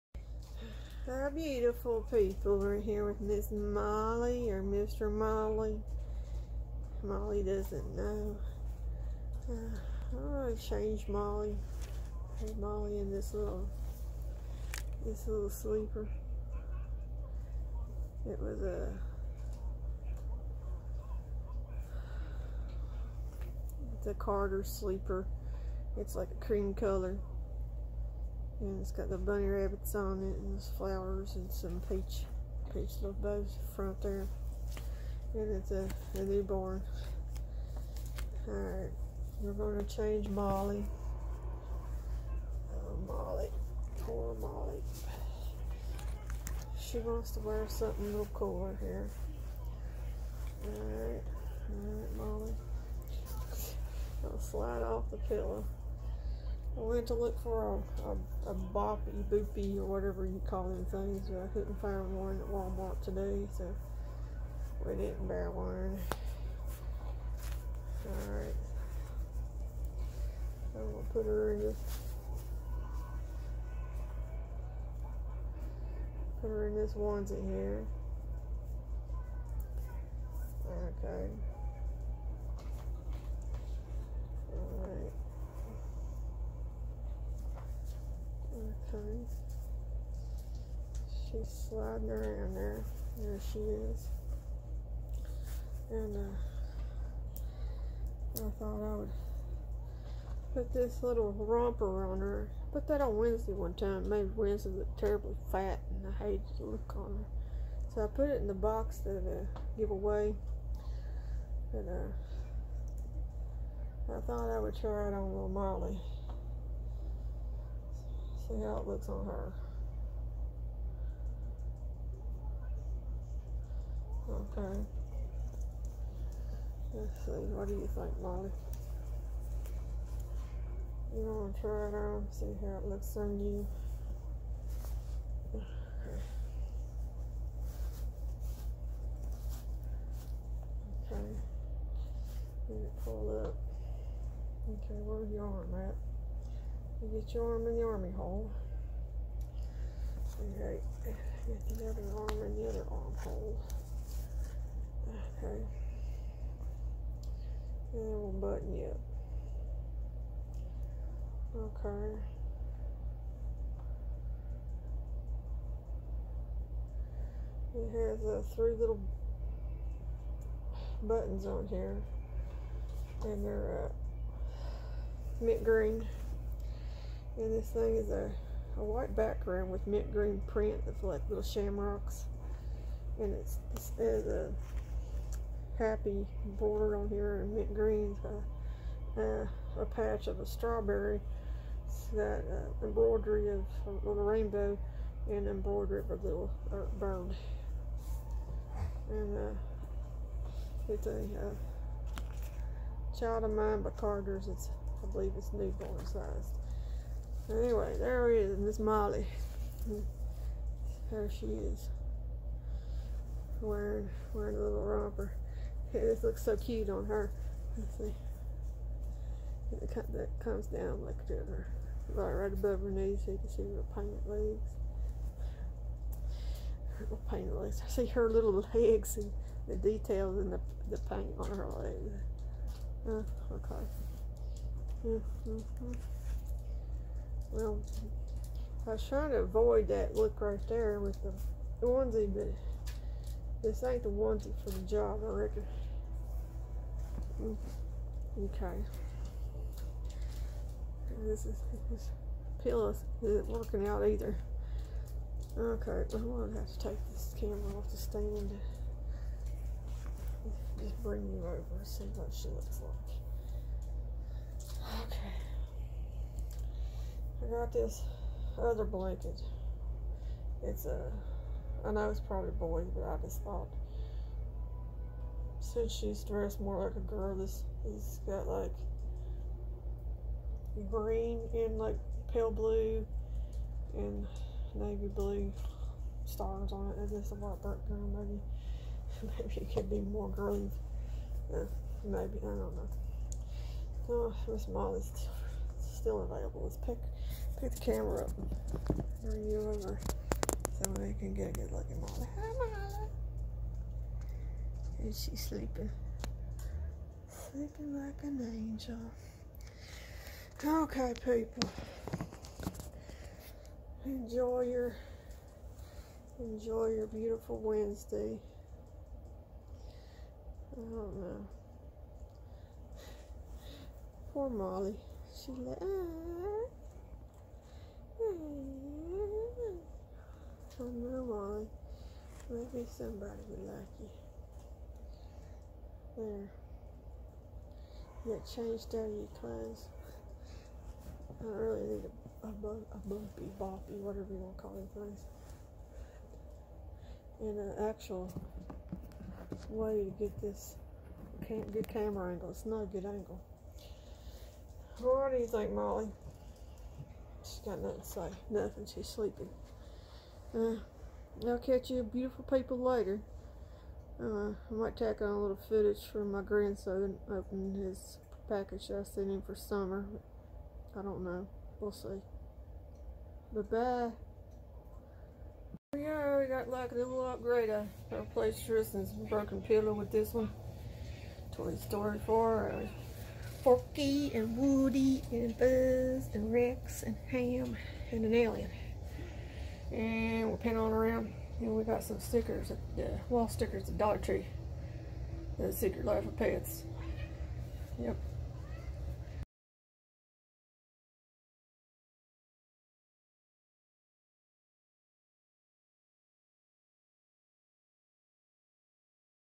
Good Golly Miss. Molly is still Available for Adoption 👶
Molly is a Bountiful Baby kit that I reborned. She is a 3/4 arm and leg baby and on a jointed doe suede body. She was painted with GHSP and sealed with a matte finish. She is stuffed with Angel Silk polypill in her body so she is really soft. She has 2 pounds of glass beads in her- 1 pound in her bottom/belly and the other pound is split between her arms, legs and head. She wears a Newborn size. If interested IM me on Instagram @ calliesroom. USA shipping only. Considered as a Boo Boo baby/beginning artist. Reasonably priced.